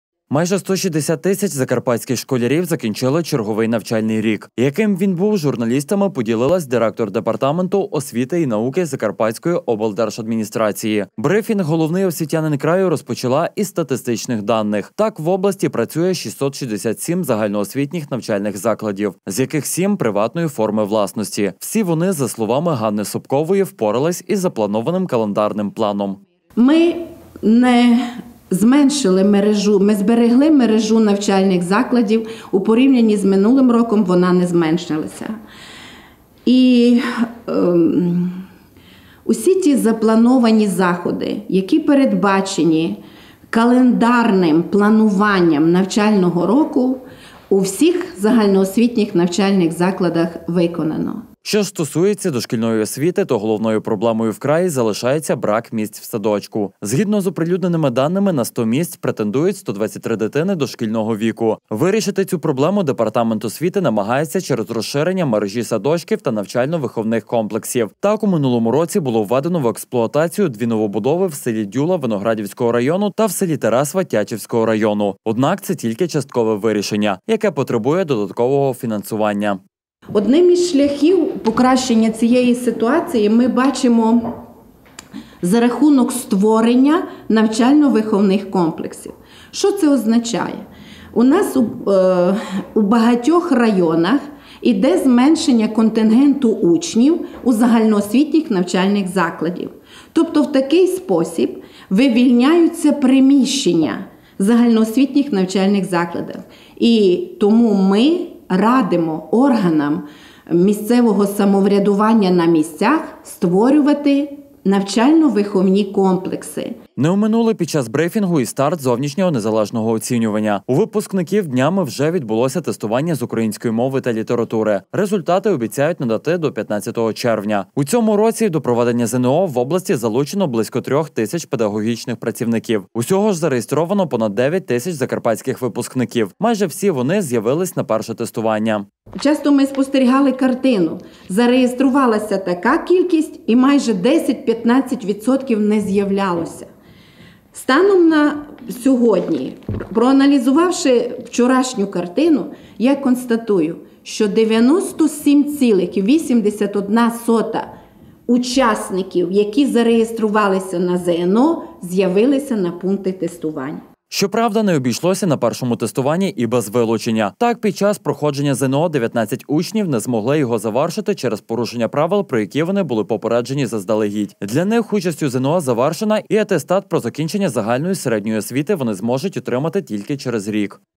Брифінг директора обласного департаменту освіти і науки
Майже 160 тис. закарпатських школярів закінчили черговий навчальний рік. Яким він був, з журналістами поділилась директор департаменту освіти і науки Закарпатської облдержадміністрації. Брифінг головний освітянин краю розпочала зі статистичних даних. Так, в області працює 667 загальноосвітніх навчальних закладів, з яких 7 приватної форми власності. Всі вони, за словами Ганни Сопкової, впорались із запланованим календарним планом.
 Що ж стосується дошкільної освіти, то головною проблемою в краї залишається брак місць в садочках. Згідно з оприлюдненими даними на 100 місць претендують 123 дитини дошкільного віку. Вирішити цю проблему департамент освіти намагається через розширення мережі садочків та навчально-виховних комплексів. Так у минулому році було введено в експлуатацію 2 новобудови в с. Дюла Виноградівського району та в с. Тересва Тячівського району. Однак це тільки частково вирішення, яке потребує додаткового фінансування.
 Не оминули під час брифінгу й старт зовнішнього незалежного оцінювання. У випускників на днях вже відбулось тестування з Української мови та л-ри, результати обіцяють надати до 15 червня. У цьому році до проведення ЗНО, в області залучено близько трьох тис. педагогічних працівників. Усього ж зареєстровано понад 9 тис. закарпатських випускників. Майже всі вони з’явились на перше тестування.
Щоправда не обійшлось на першому тестуванні й без вилучення. Так під час проходження ЗНО, 19 учнів не змогли його завершити через порушення правил, про які вони були попереджені заздалегідь. Для них участь у ЗНО завершена, й атестат про закінчення загальної середньої освіти вони зможуть отримати тільки через рік.